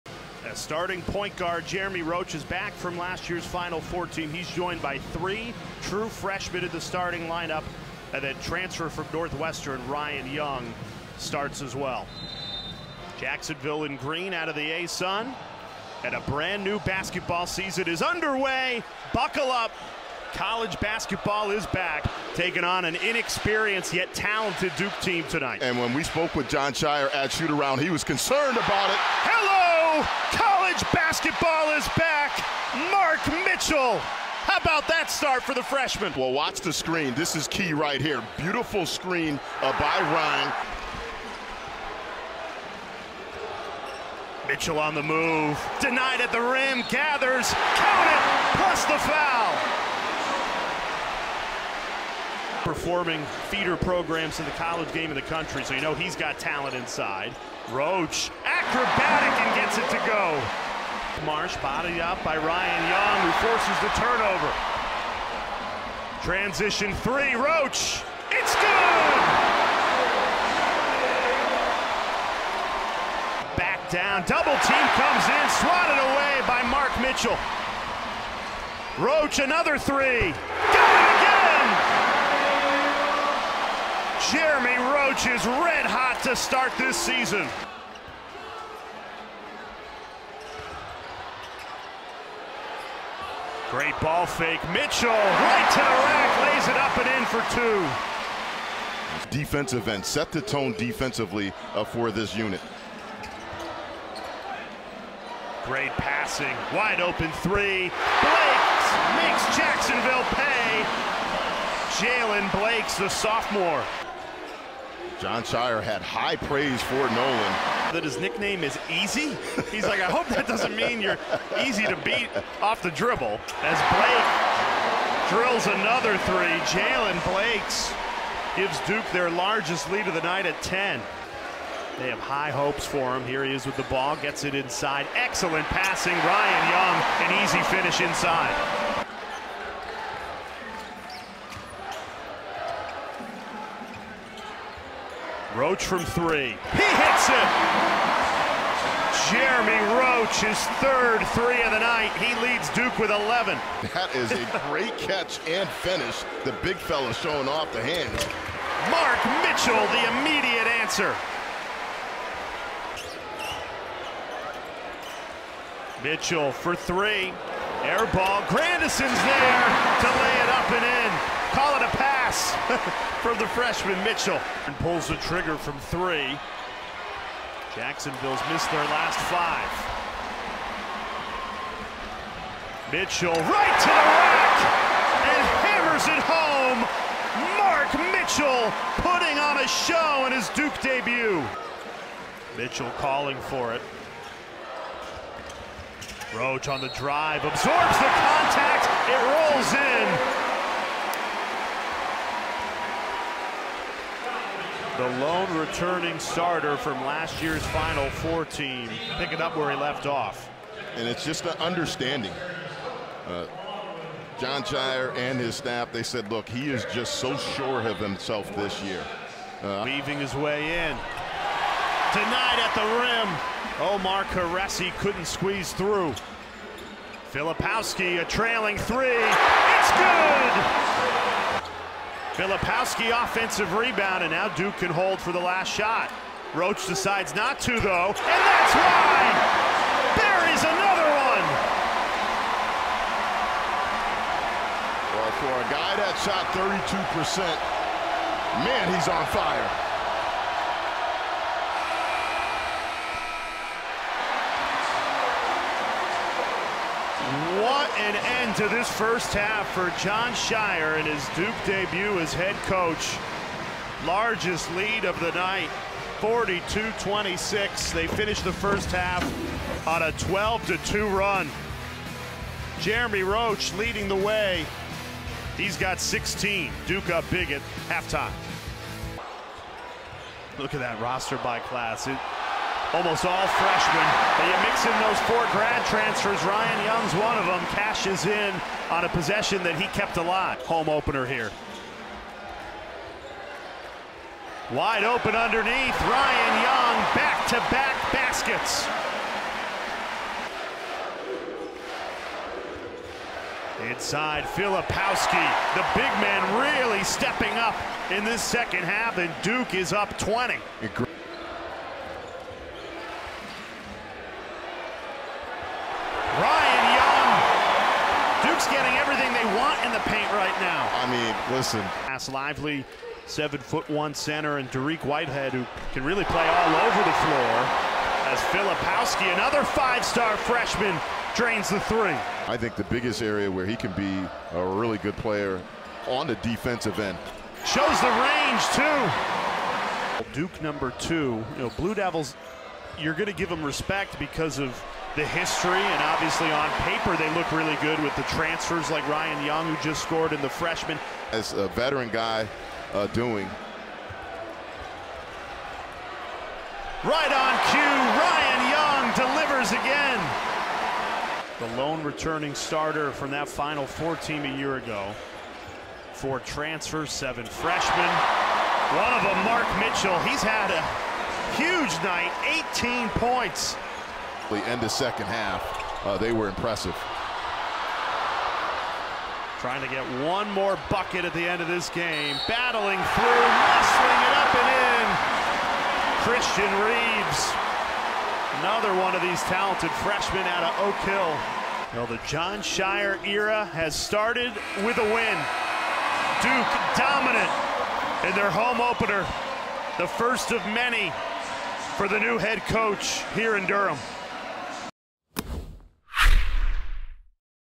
Duke 71, Jacksonville 44 | Every Bucket (11/7/22)
Every bucket from Coach Jon Scheyer's 1st win as Duke Head Coach!